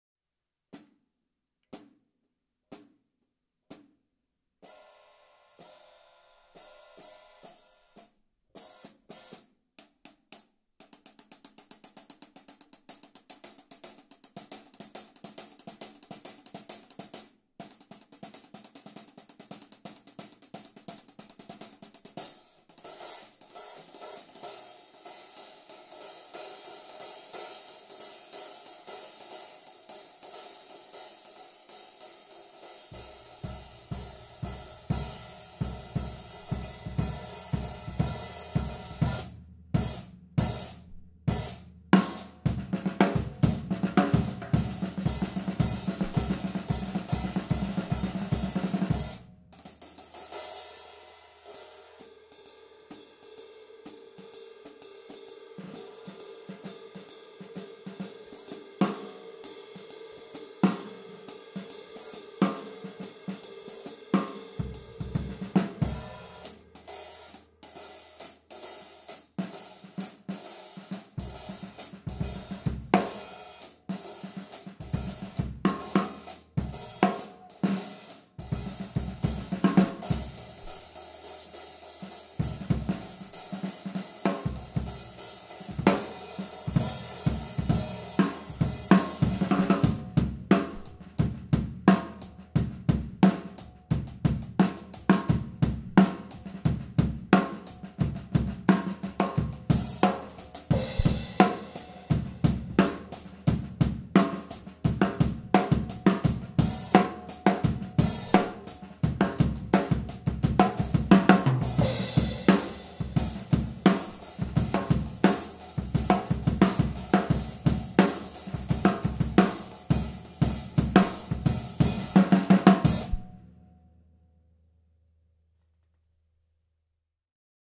Bosphorus 14" Traditional Dark Hi Hat Cymbals - 890/1070g (T14HD-1083117FF)
*SOLD* Bosphorus 14" Traditional Dark Hi Hat.

Other gear used in this video:
• Cymbals (From drummers L to R): Bosphorus 18" Master Vintage Crash Cymbal, Bosphorus 20" Master Vintage Ride Cymbal